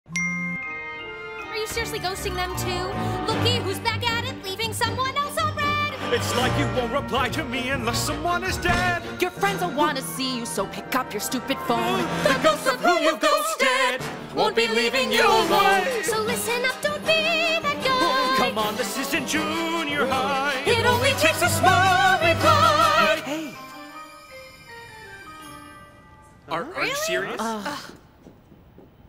When friends you've GHOSTED come to haunt you!👻
Starring Max Nussbaum, Gabriella Whiting, Sam Borthwick, Feras Halabi
Lyrics by Julia Gannon
Music & Orchestration by Alonso Pirio